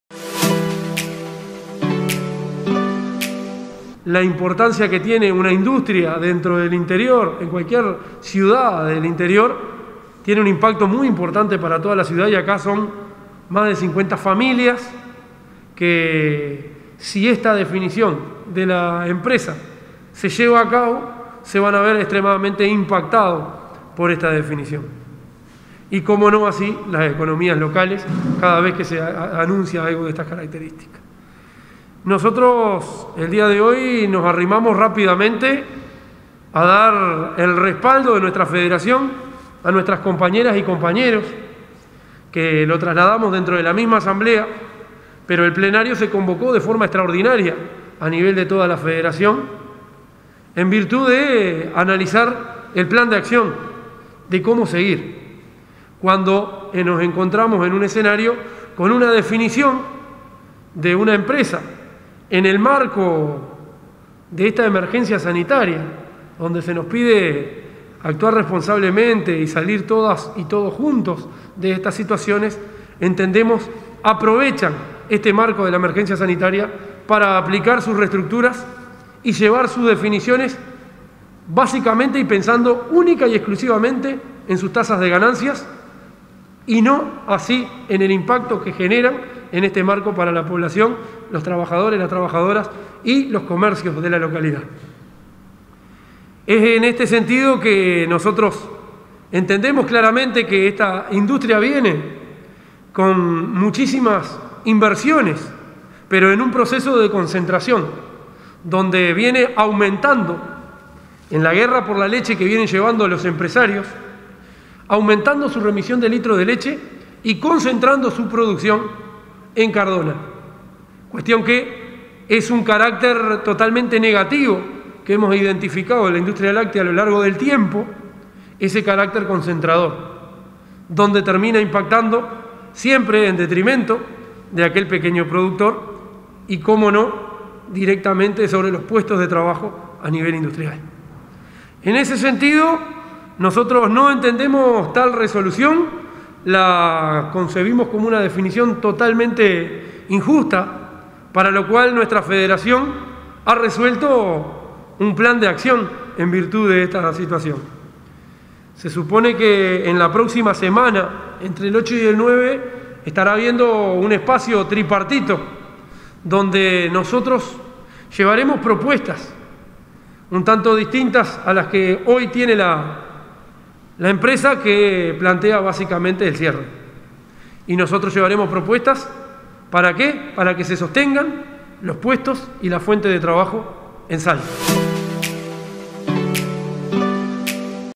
Trabajadores de INDULACSA Salto movilizados por anuncio de cierre de la planta
Trabajadores de la empresa INDULACSA se encuentran movilizados tras el anuncio del cierre de la empresa por parte de los propietarios. Son más de 50 familias que quedan desempleadas, y debido a esa razón, Enrique Méndez de la Federación Lactea del Pit-Cnt, informó que en la próxima semana se "estará llevando a cabo un espacio tripartito, donde nosotros llevaremos respuestas, para que se sostengan los puestos de trabajo en salto".